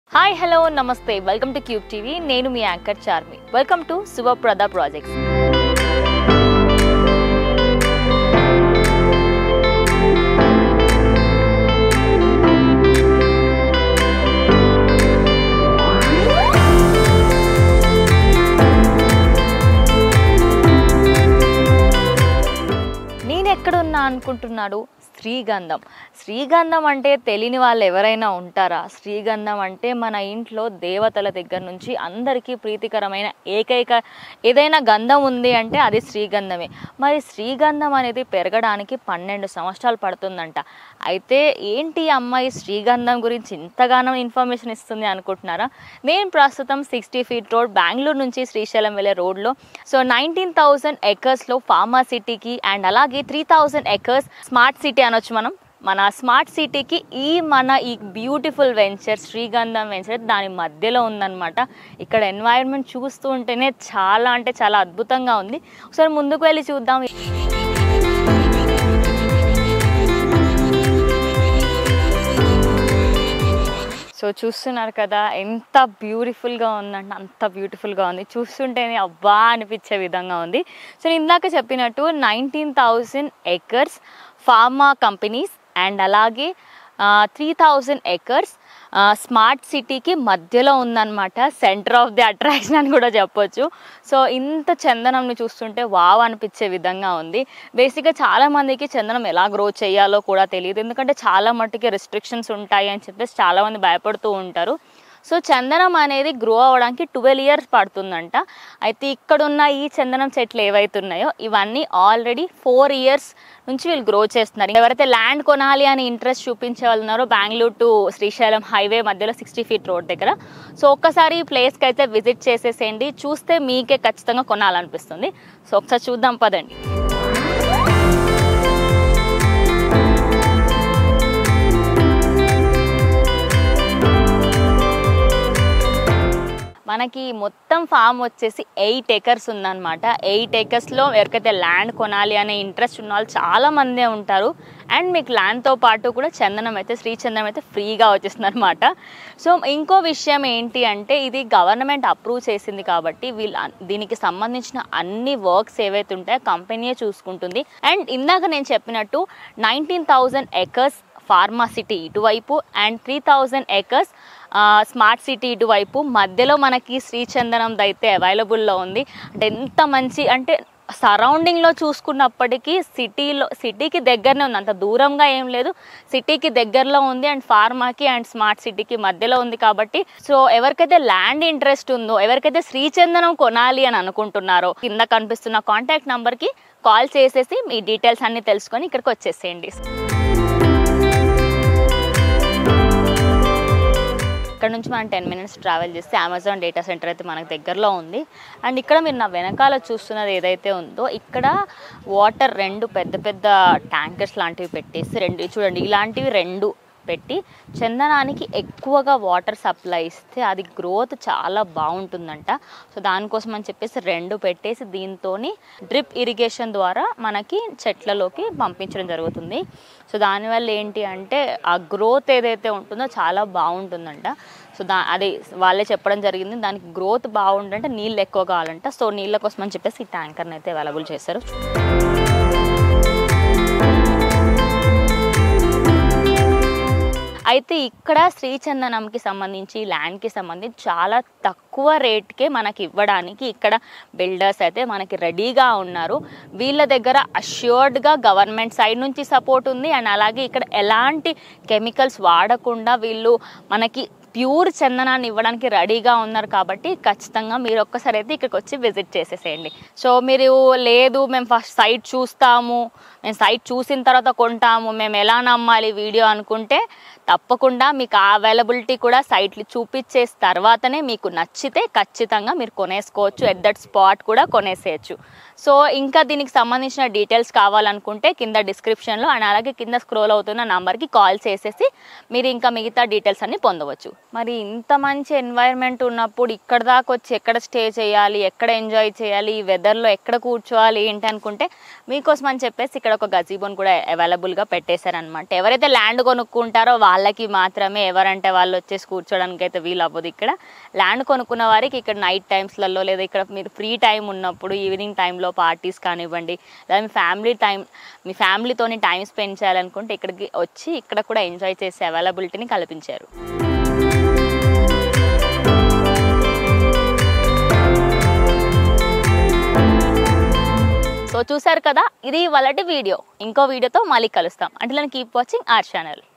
కోట్లు విలువ చేసే సాండల్ వుడ్ ఫ్రీ: Sree Gandham Land Ventures | More Benefits With Small Investment
కోట్లు ఖర్చు చేసే సాండల్ వుడ్ ఫ్రీ: Sree Gandham Land Ventures | More Benefits With Small Investment | Shuba Pradam Projects

అతి తక్కువ పెట్టుబడి, అతధిక లాభం 4 సంవత్సరాలు పైబడి పెరిగిన శ్రీగంధం చెట్ల ప్లాట్లు. MyHome Smart City, Amazon, Johnson & Johnson and P&G కంపెనీస్ దగ్గర, స్పాట్ రిజిస్ట్రేషన్ & మరిన్ని వివరాలకోసం కోసం వెంటనే కాల్ చేయండి శుభప్రదం ప్రాజెక్ట్స్ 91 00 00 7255